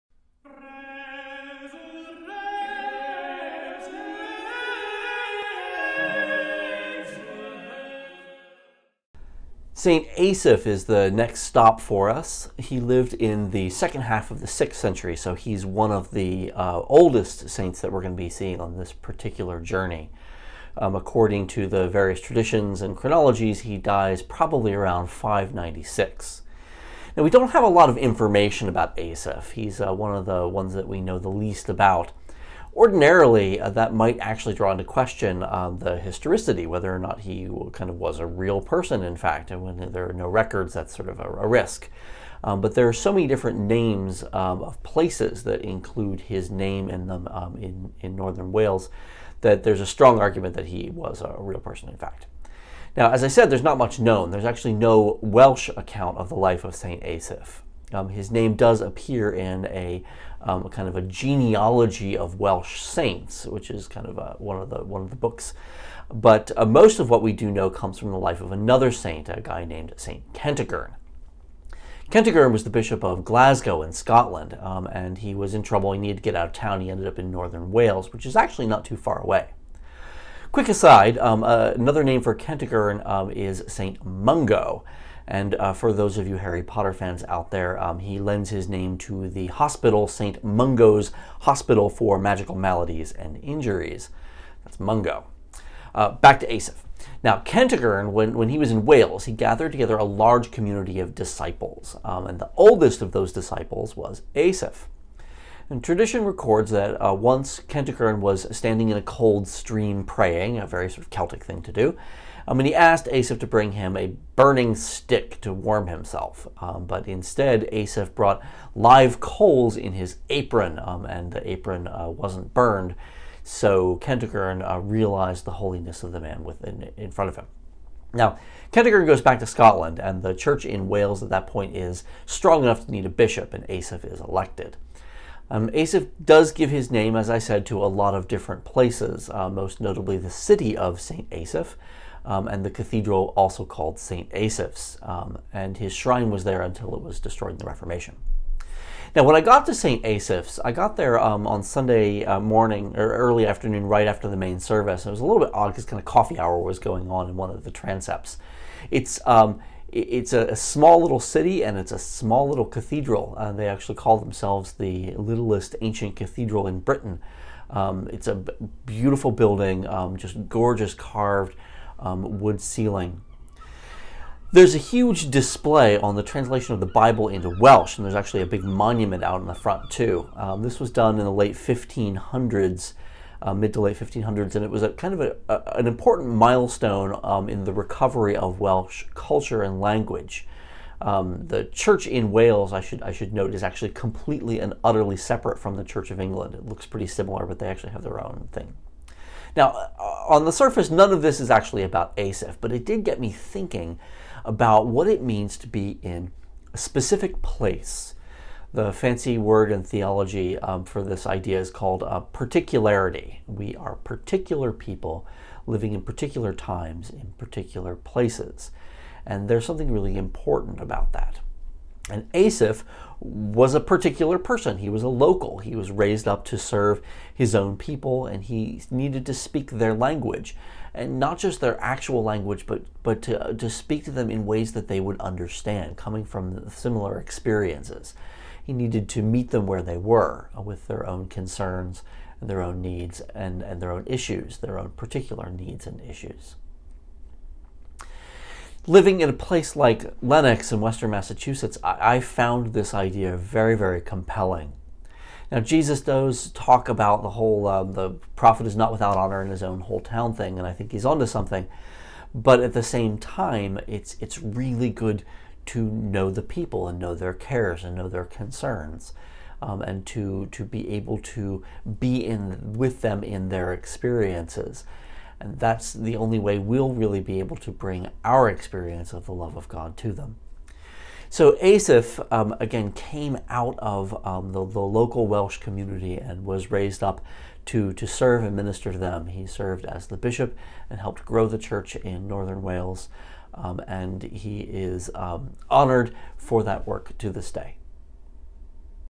Pilgrimage - Saint Asaph of Wales
Michael Tuck, a parish priest from Western Massachusetts (Lenox) will be visiting shrines and holy places around England. Prayers and more information can be be found on his blog